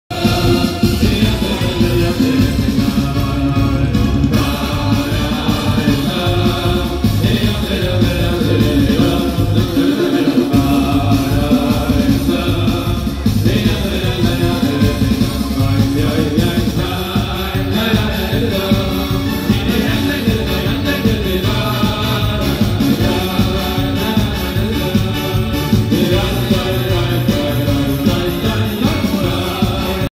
Boy Juggling With Fire In Front Of Thousands At Tosh-Pupa Sheve Bruches
Follow, The Yiddishe Boys Platform

Telegram • T.me/YiddisheBoys